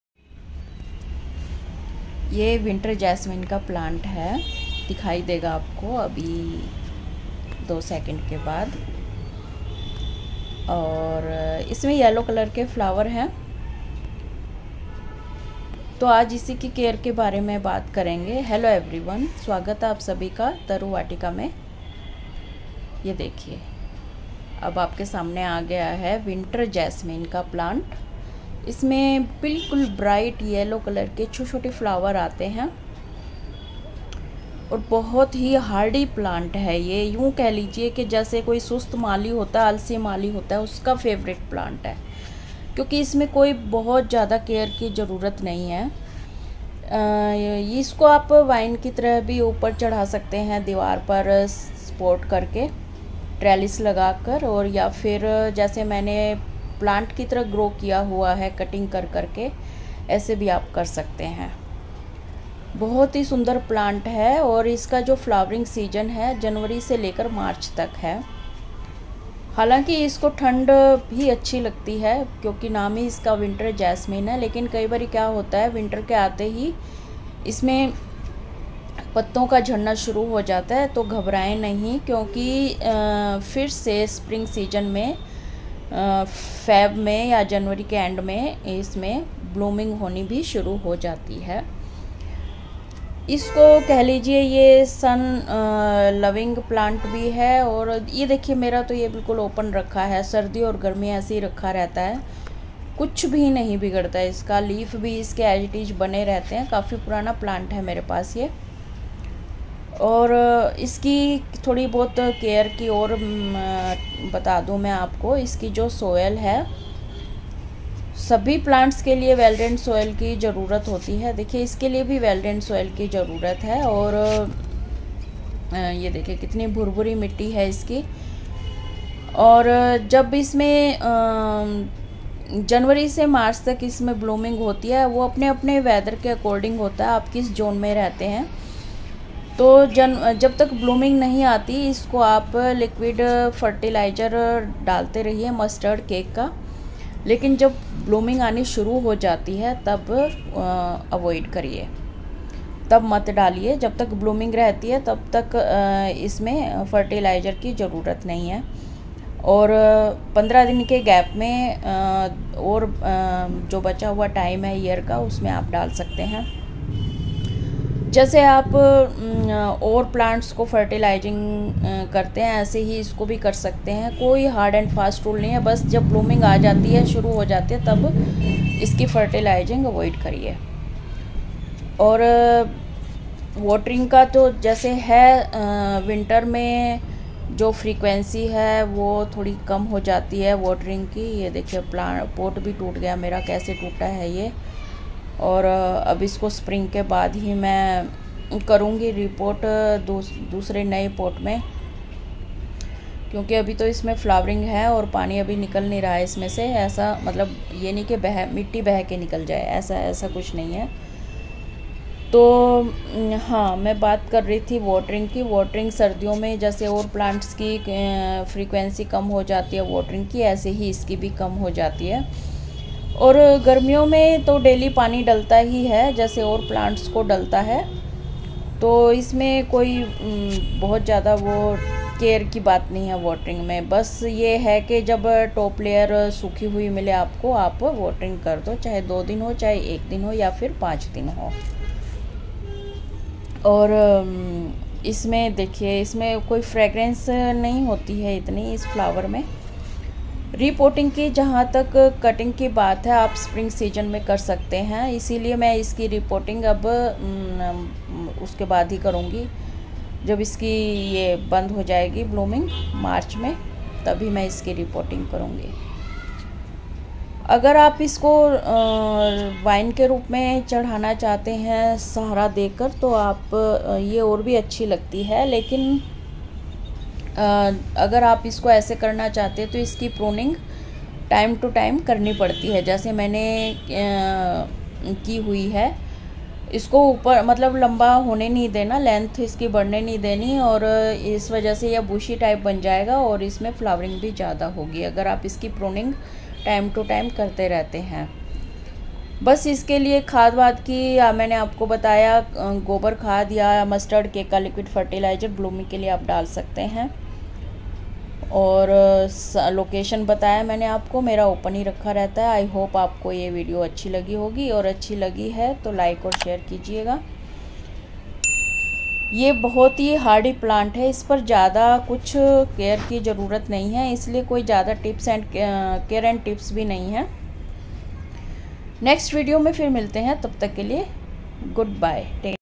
Look after#Care of Winter jasmine#Taru Vatika 250-How to look after of Winter Jasmine?
Welcome to all my lovely friends 🙏

In this video I will show you how to do care of winter jasmine?It must grow in well drain soil.It blooms from January to March.It is hardy plant free of infestation 👍 Enjoy this video.